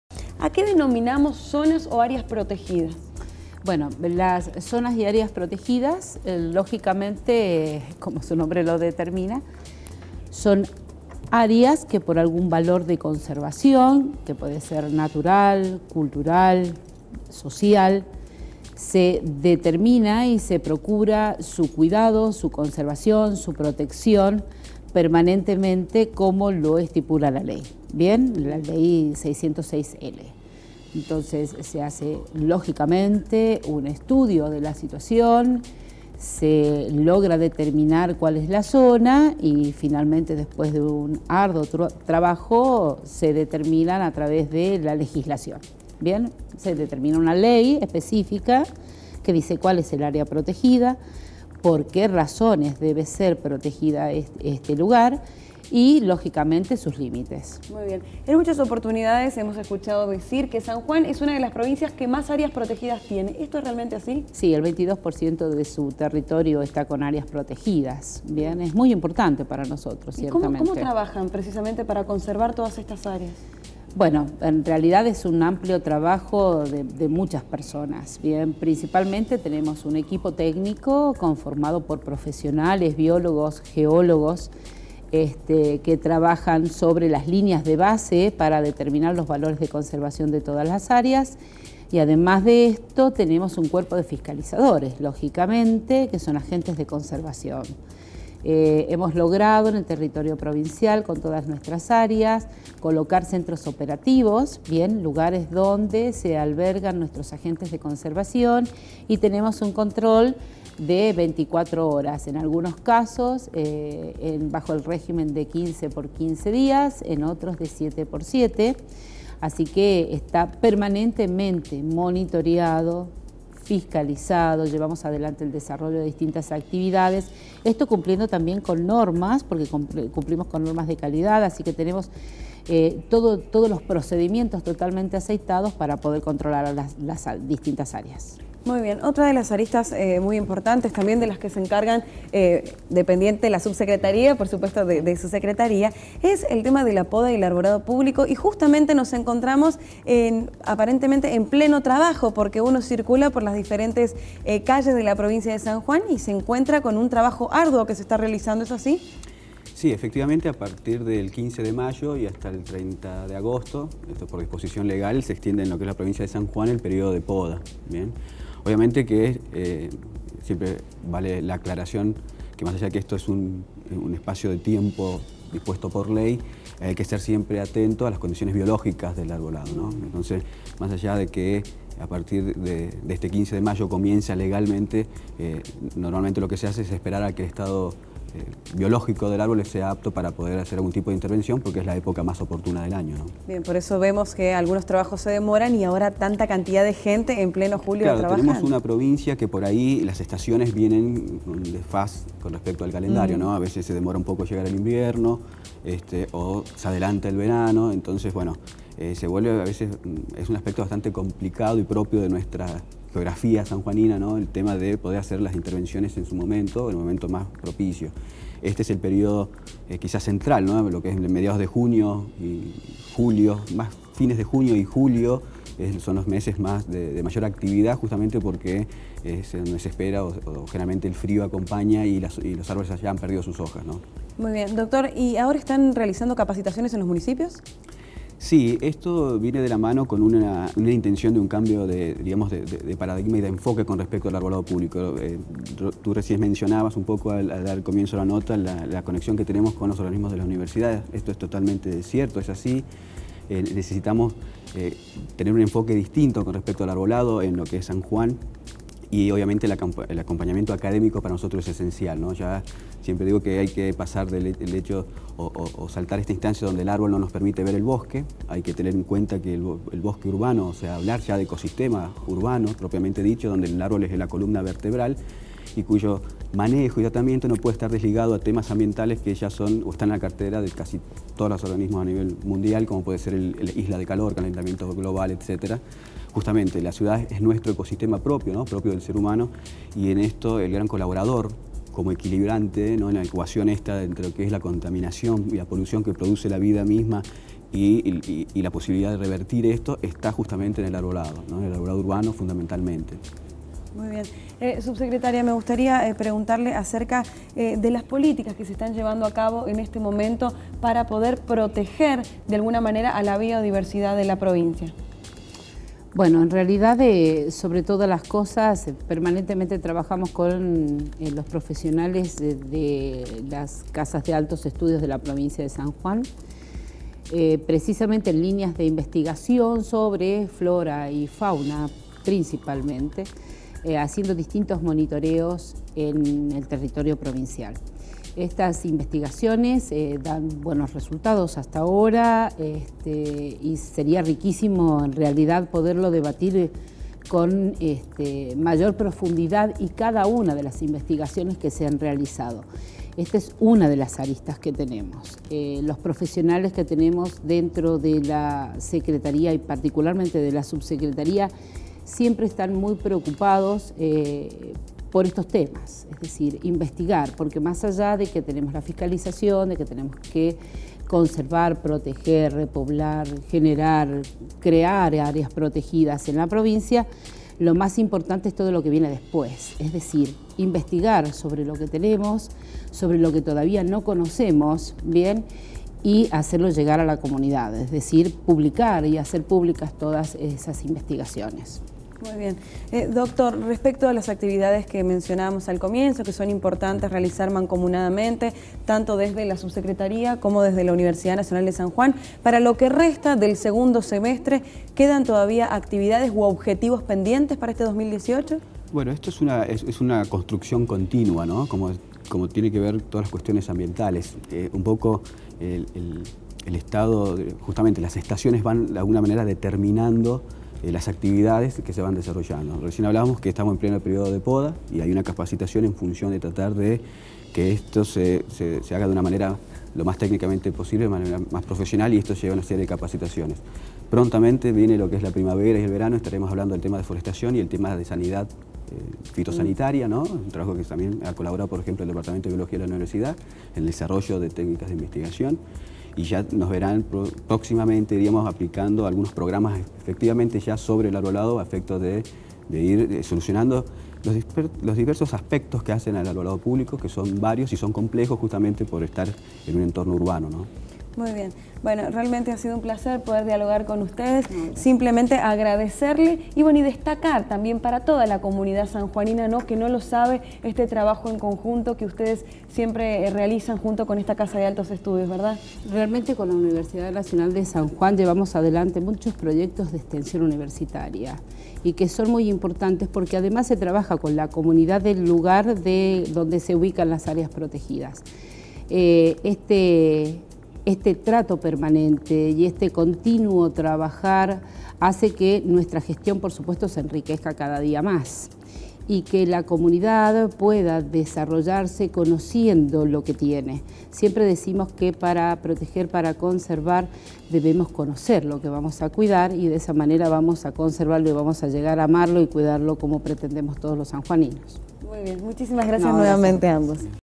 Universidad en VIVO- Zonas protegidas de San Juan
Entrevista a Alejandra Cano de la Subsecretaría de Conservación y Áreas Protegidas y a Héctor Bustamante, director de Arbolado Público.